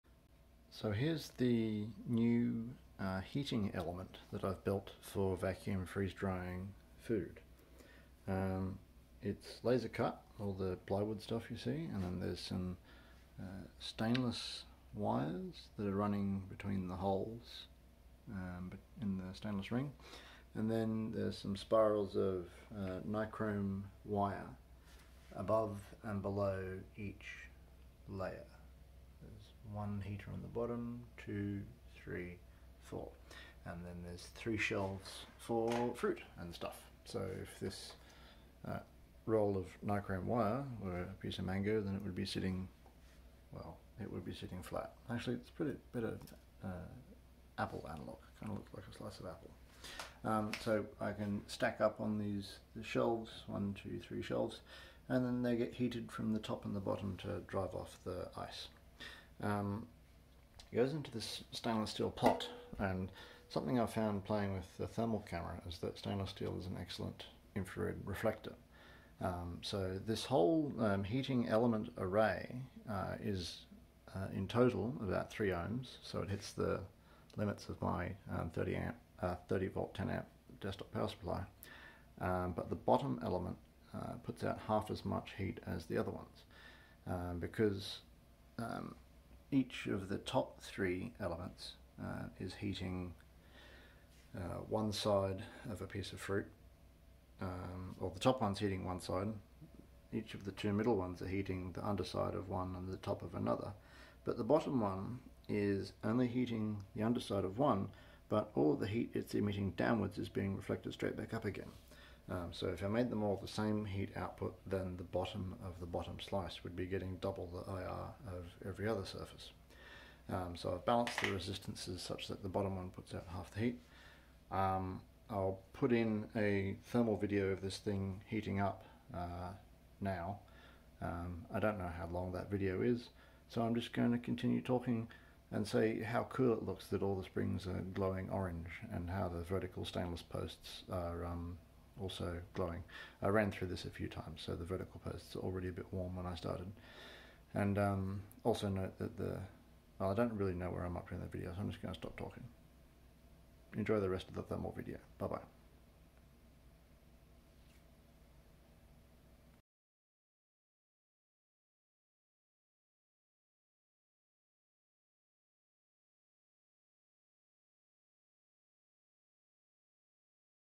Vacuum freeze dry heater element rack
This is a set of shelves and heating elements for vacuum freeze-drying fruit. The heating elements are above and below the shelves that hold the fruit slices.